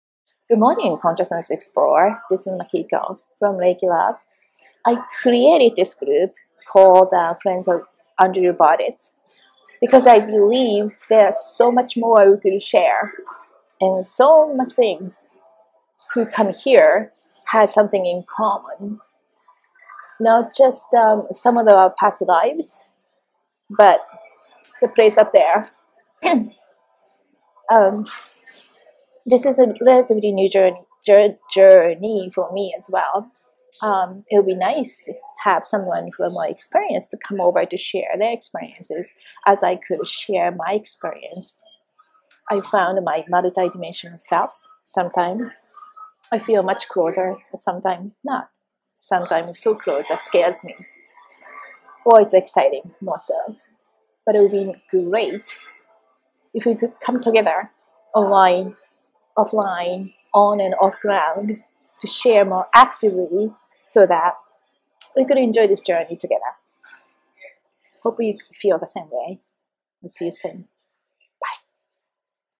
Greetings and welcome message from makiko "Friends Who Like Andrew Bartzis"
This video is about Greetings and welcome message from makiko new Facebook discussion group "Friends Who Like Andrew Bartzis"
Please join in this fb group or use the following link to get the event details.